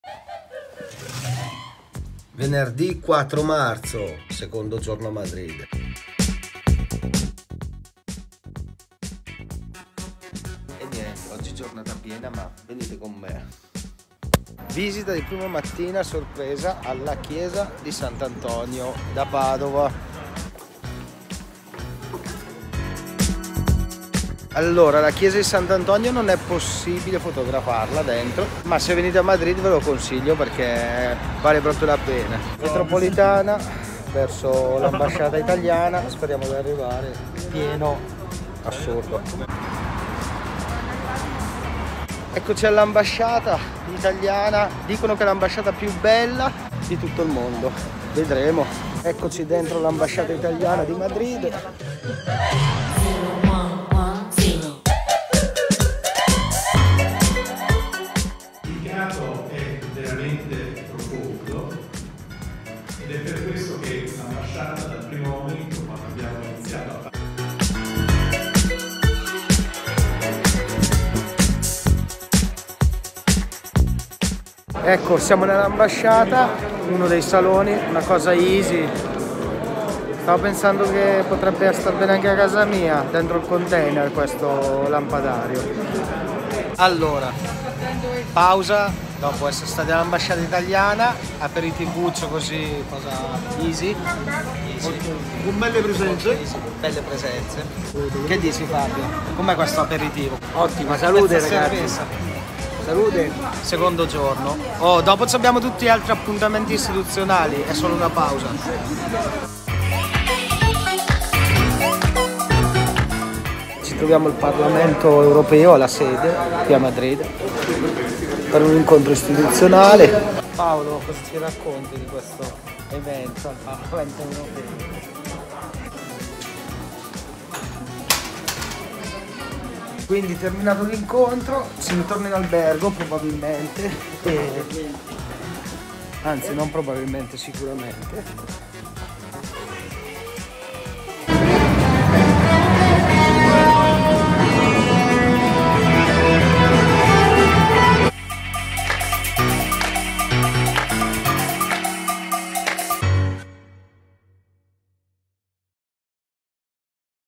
(Ep.4) IL VIAGGIO: MADRID, visita AMBASCIATA ITALIANA & PARLAMENTO EUROPEO!NORCIA SAN BENEDETTO 2022
🌍 IL VIAGGIO: INTRO - la FIACCOLA di SAN BENEDETTO, CELEBRAZIONI 2022: NORCIA, MADRID, SANTIAGO

- OGGI EPISODIO 4 ( se non hai visto i precedenti GUARDA la PLAYLIST!!! )... IN ambasciata italiana, e al parlamento EUROPEO!!!

- vi voglio portare con me, nel viaggio inserito nelle celebrazioni 2022 benedettine di Norcia, Subiaco e Cassino…  in cui porteremo la fiaccola di San Benedetto, patrono d’Europa, con il messaggio di pace, che mai come in questo momento, risulta attuale!!!

🌍 Quest’anno andremo in Spagna, a Madrid e Santiago de Compostela!!!

- venite con me???

- e seguimi anche su INSTAGRAM: “lu_poru_prete”